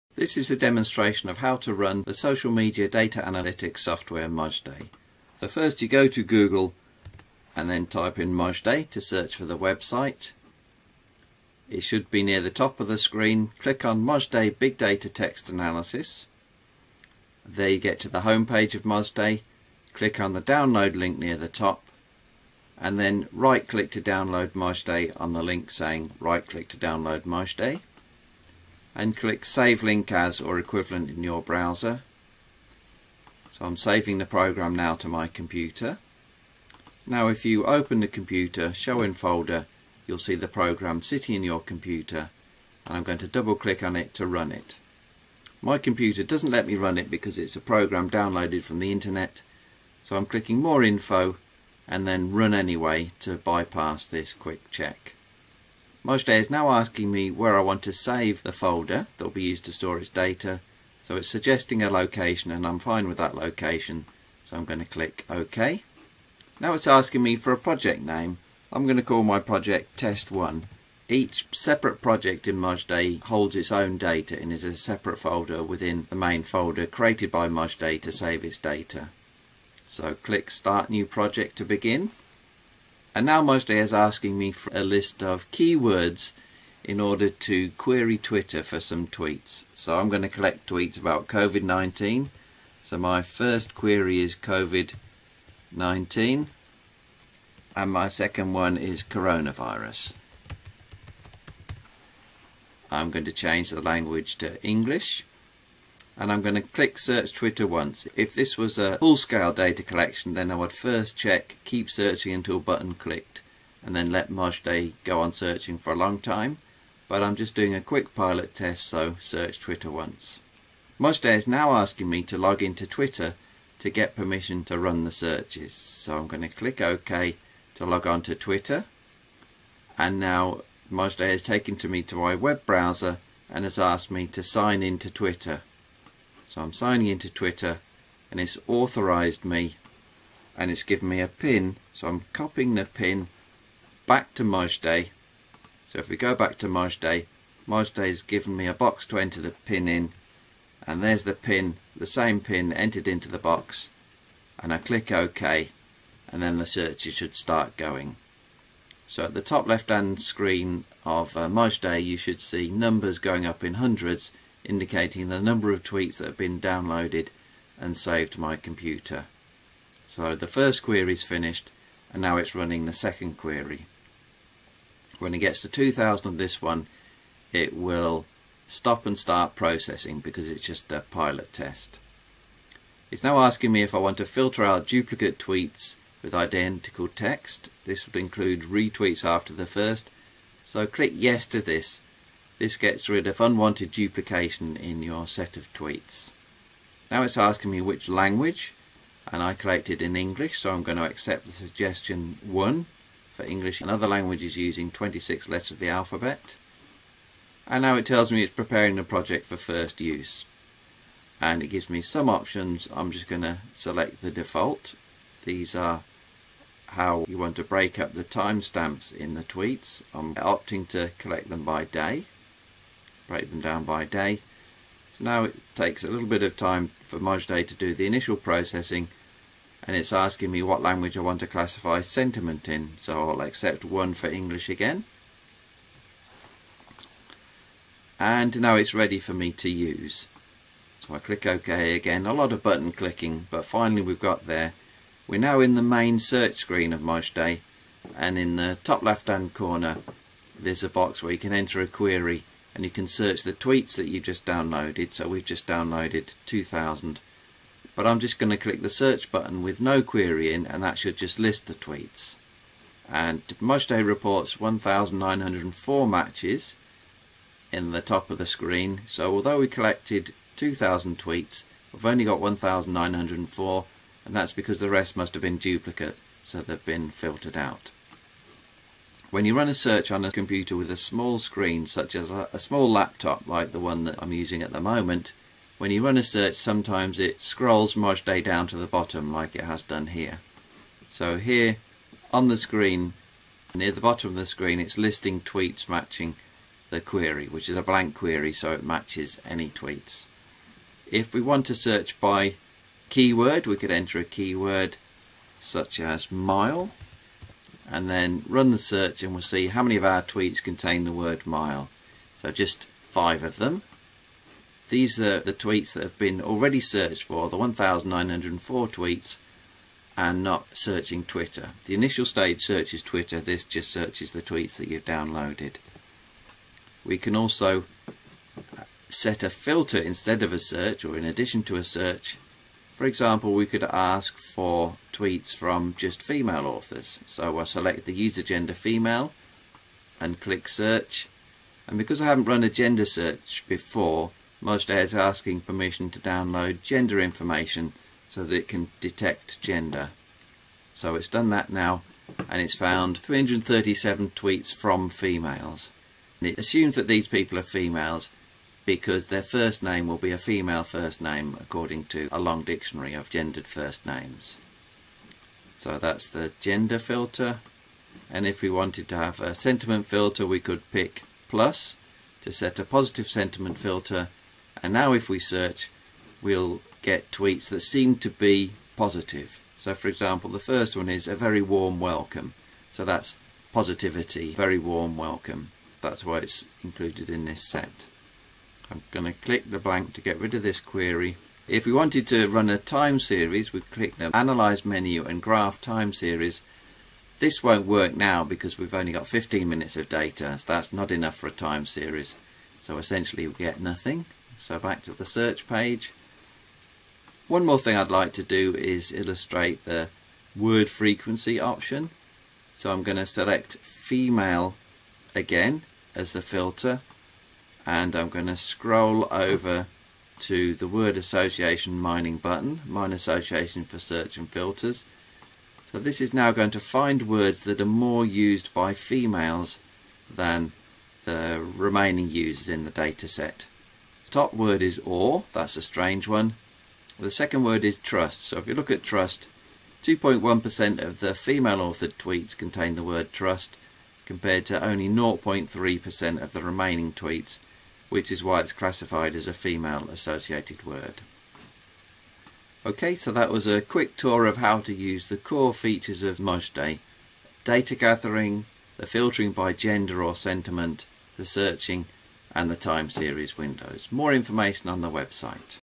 Mozdeh social media analytics with Twitter - 8 minute demo
This is an introduction to the free Windows social media analytics software Mozdeh, explaining how to download it, run it, use it to gather tweets and then search, filter, graph and word frequency analyse those tweets.